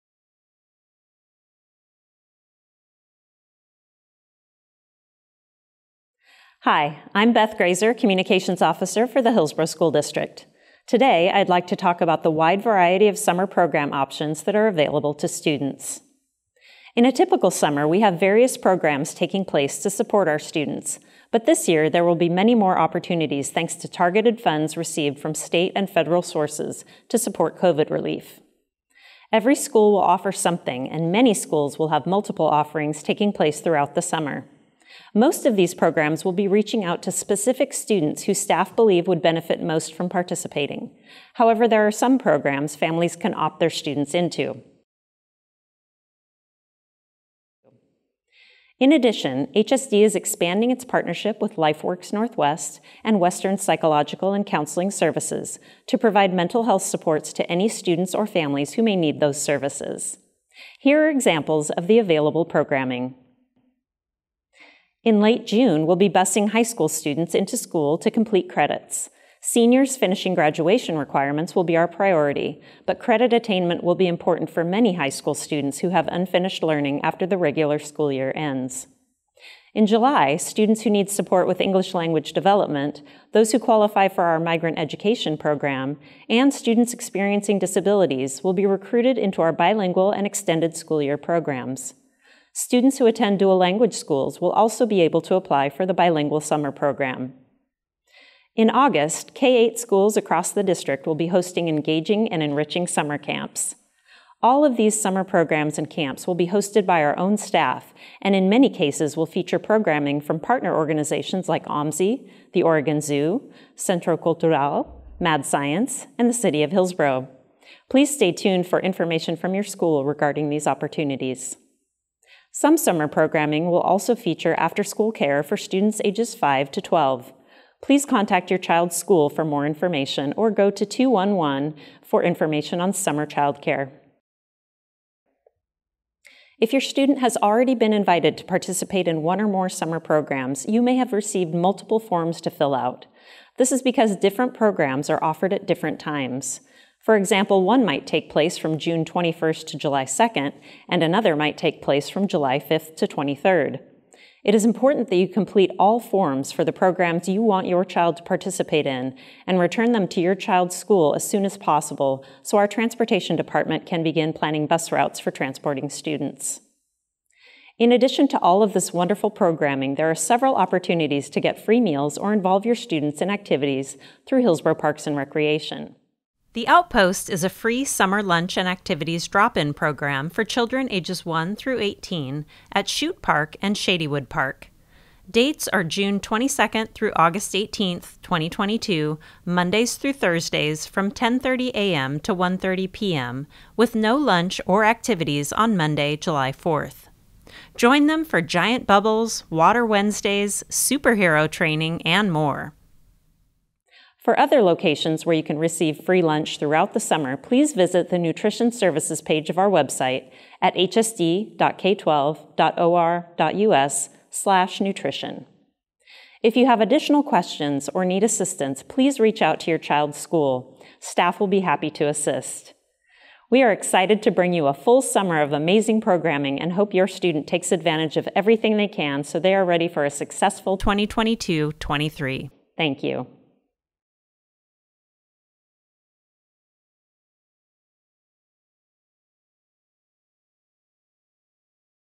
2022 Summer Program Options, Hillsboro School District
In a typical summer, we have various programs taking place to support our students, but this year, there will be many more opportunities thanks to targeted funds received from state and federal sources to support COVID relief. Every school will offer something, and many schools will have multiple offerings taking place throughout the summer. The Outpost